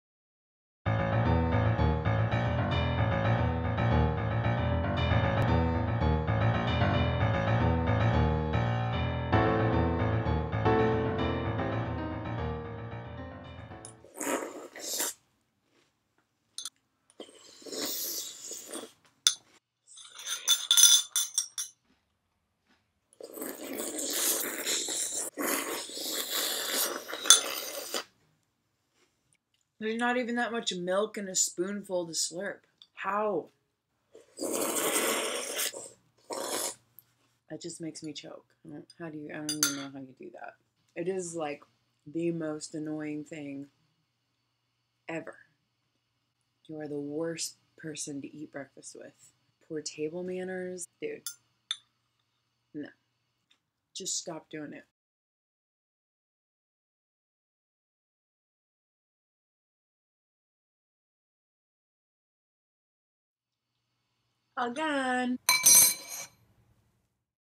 Cereal Slurp. No.
I love you, but if we're eating breakfast and you start slurping, shoveling or smacking, I'll have to ask you to leave my presence.